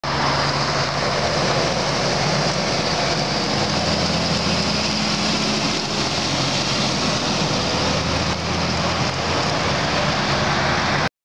Mainland Floral, on the road!
One of the Mainland Floral trucks, on the road to make deliveries to some our customers in Washington State.